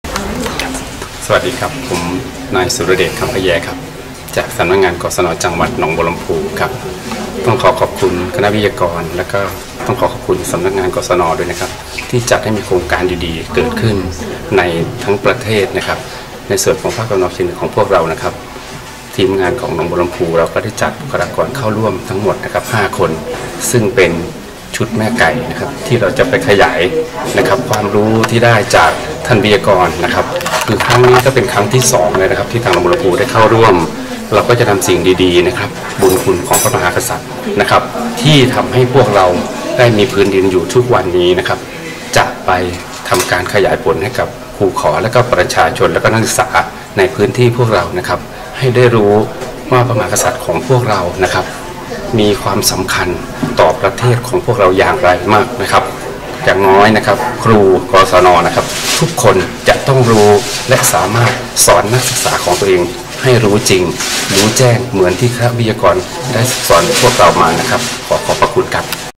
บทสัมภาษณ์ ครูกศน แกนนำประวัติศาสตร์ ภาคอีสานตอนบน คนที่9
นายกองตรีธารณา คชเสนี และว่าที่ร้อยตรีน้ำเพ็ชร คชเสนี สัตยารักษ์ และทีมงานอีก8คน ได้จัดโครงการสร้างครูต้นแบบ "ค่ายประวัติศาสตร์ชาติไทย" ให้กับครูกศน.จังหวัดละ 5 คน โดยรุ่นที่4 จัดวันที่ 7-9 ธค 60 กลุ่มเป้าหมายคือครูกศน. 12 จังหวัดภาคอีสานตอนบน